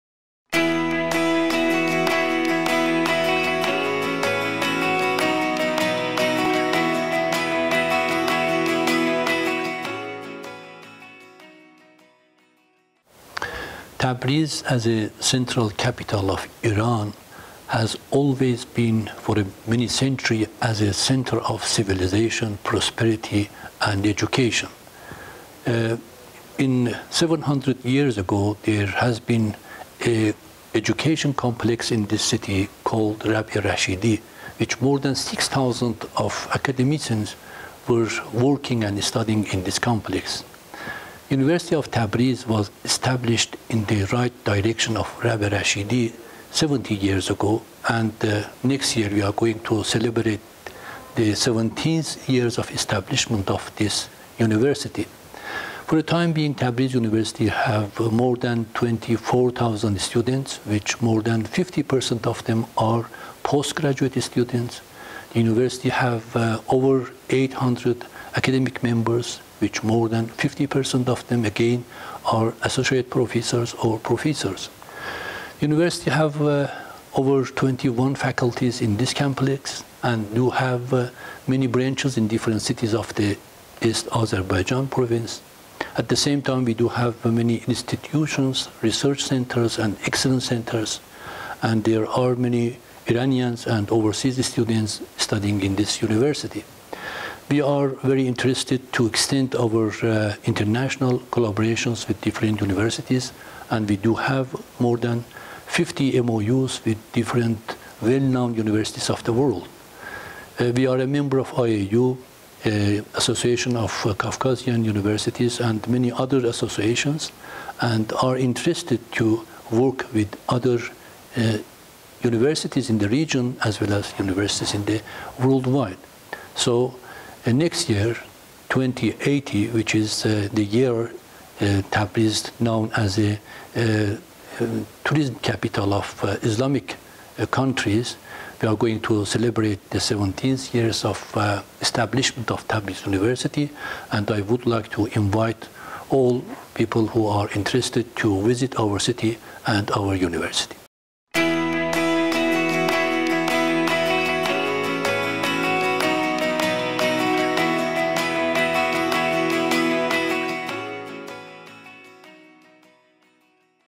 Hear from Dr. Mohammad Reza Pourmohammadi about Tabriz city and University of Tabriz
Hear from Dr. Mohammad Reza Pourmohammadi, President of University of Tabriz, about Tabriz city, University of Tabriz and Tabriz 2018